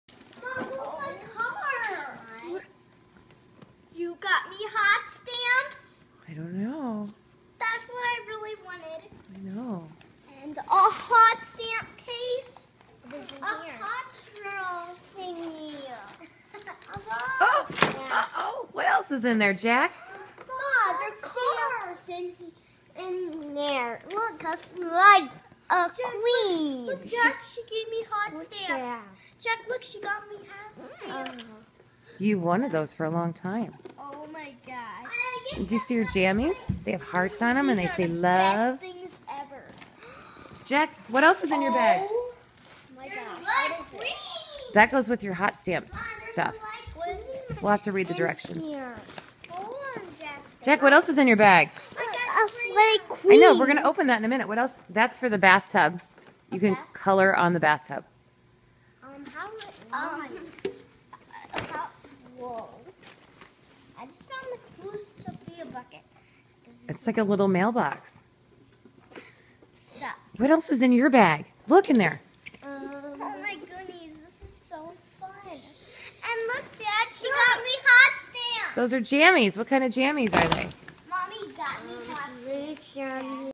My lil valentines
Ava and jack opening their loot ❤️